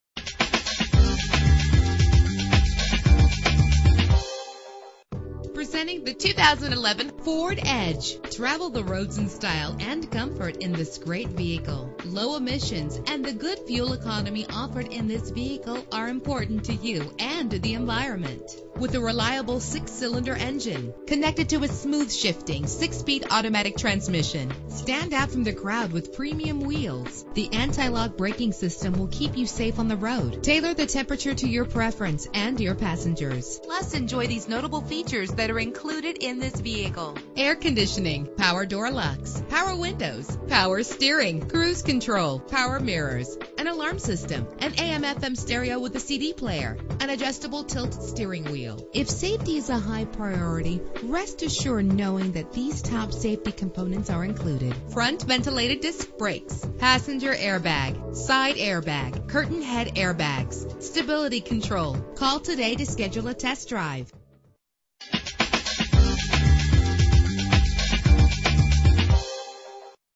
2011 Ford Edge - Cicero NY
Year: 2011
Make: Ford
Model: Edge
Trim: SEL
Engine: 3.5 liter 6 cylinder 24 valve
Transmission: 6-Speed Automatic
Color: Maroon
Mileage: 23850
Address:
5885 E Circle Dr